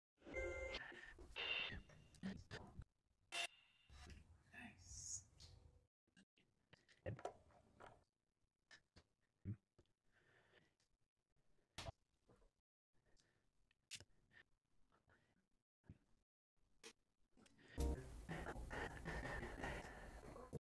How to Build Your Own Acoustic Panels but so good there is no sound (0 manual editing)
Just something quick and dumb I was testing out, uses Carykh's video editor

Do you for some reason want me to edit your videos?